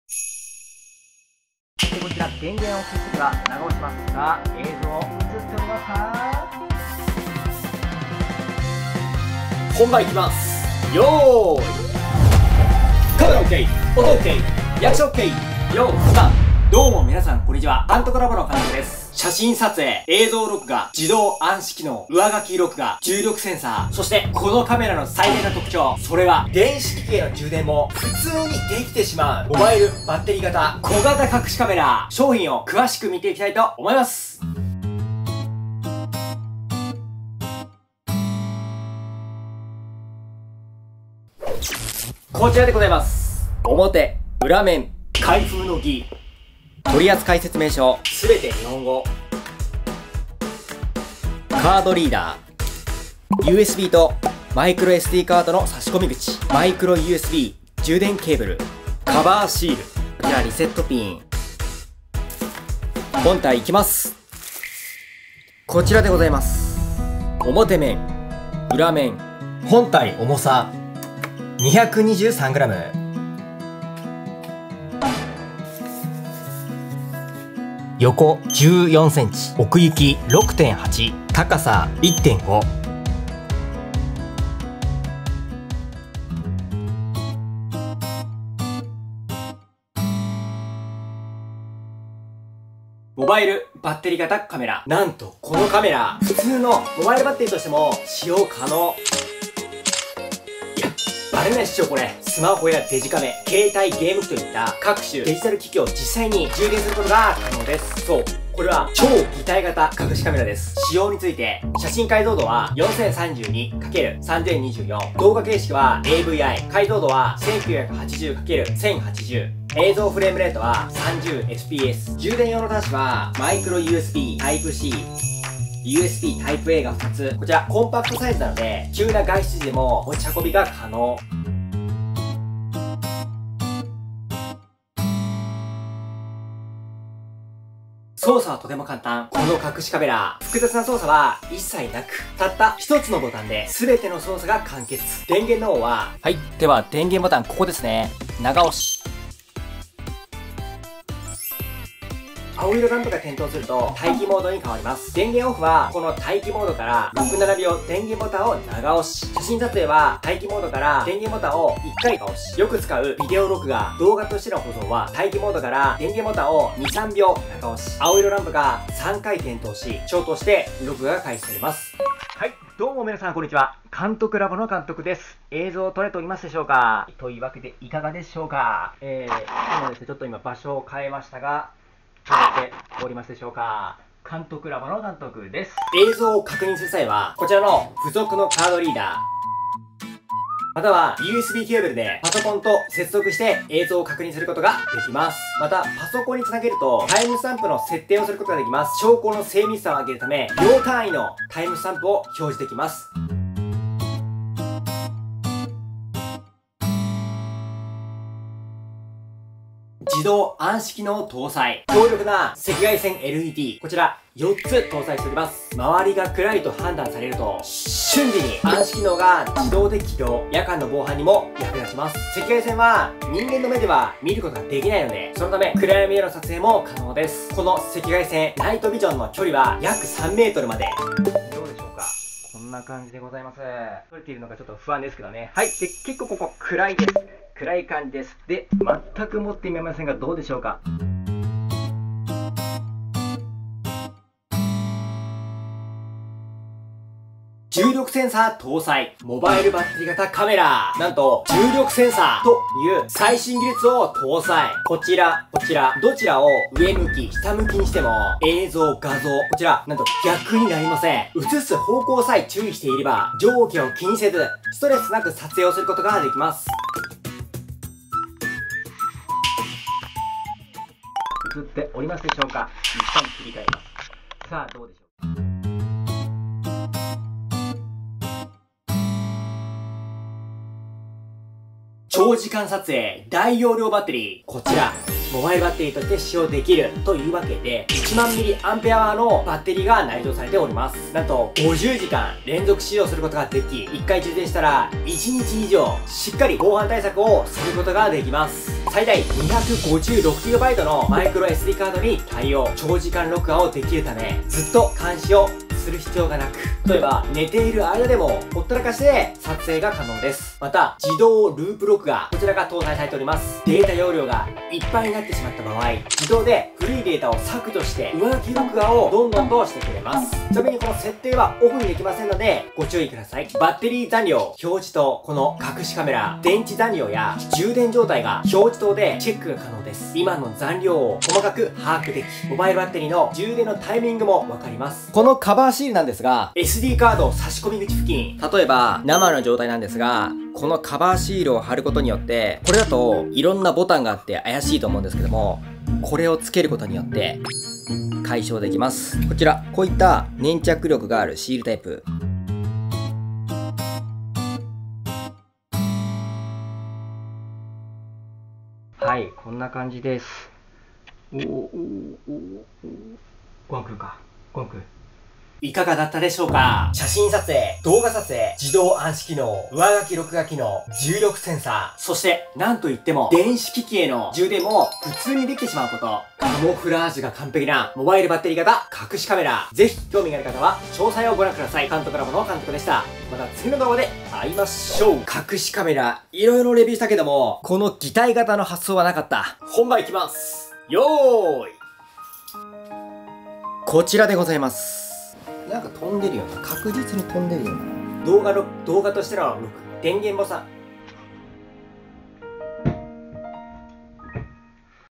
《悪用厳禁》一度見ただけでは絶対わからなーい！モバイルバッテリとしても普通に使えて、重量センサー搭載している隠しカメラが凄い【Generic 】
Amazon商品サイト（Generic / バッテリ型隠しカメラ）

普通にモバイルバッテリとしても使用できるカモフラージュ隠しカメラを紹介。

動画解像度「1920×1080pix」
重力センサー搭載でカメラの向きさえ注意すれば、上下に気にせず撮影ができます！

《関連動画》
ー